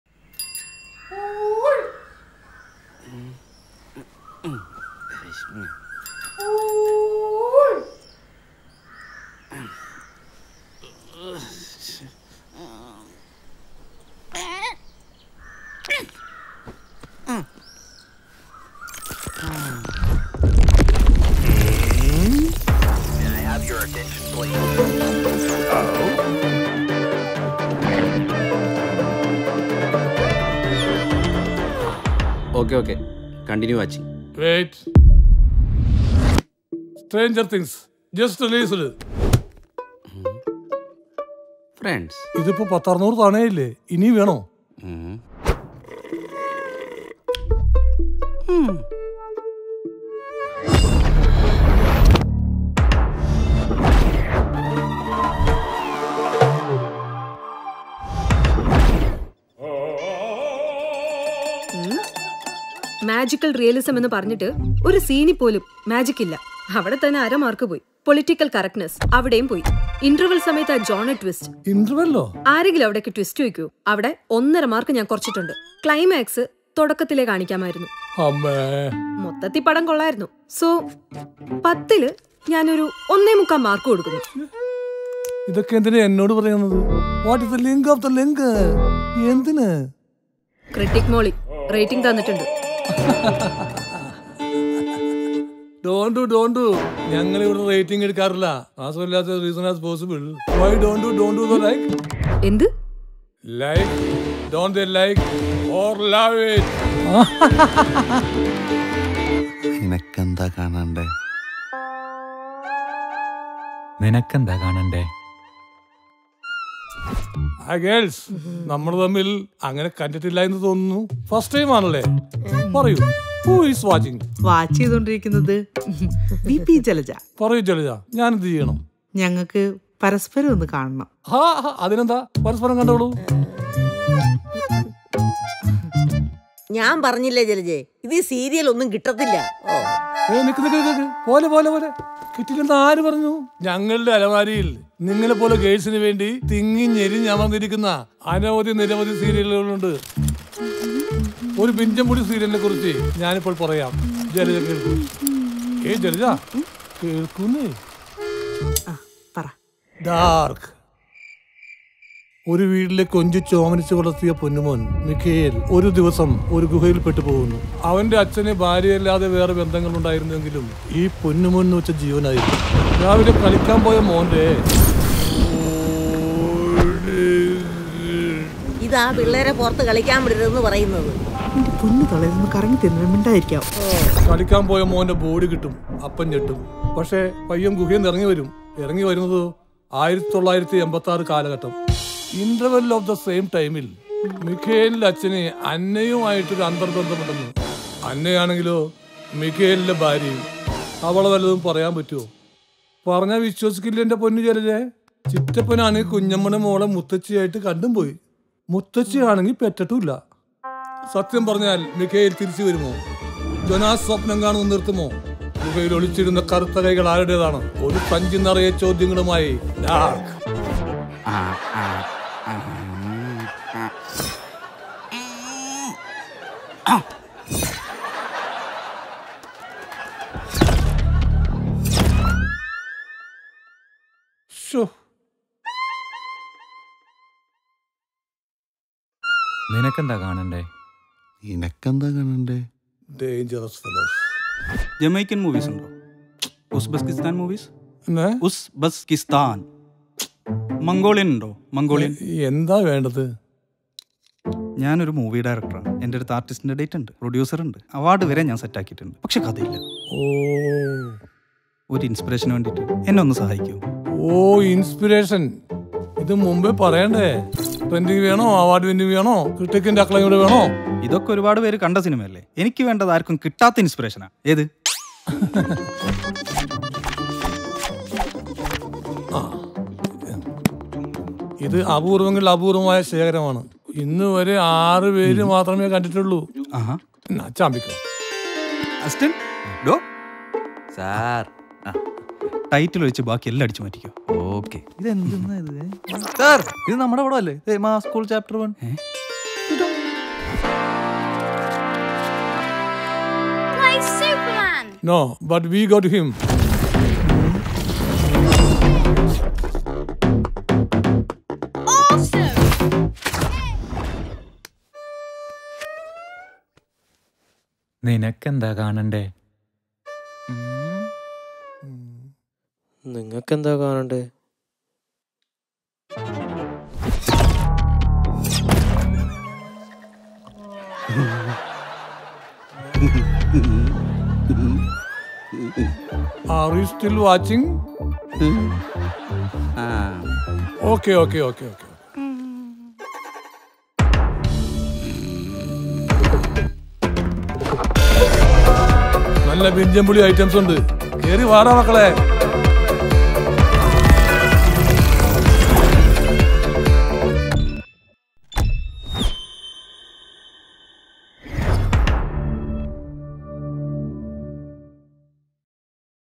What If Salim Kumar Was Netflix? | Malayalam Sketch | Netflix India
We finally found the answer to the golden question: Ninaku entha kaanande?
We want to watch Salim Kumar as Netflix! 😎

Don’t forget to tell us if you like, don’t they like or love it! ❤️

Production House: Mindstein Studios
Producer: Vyshakh Sudhakaran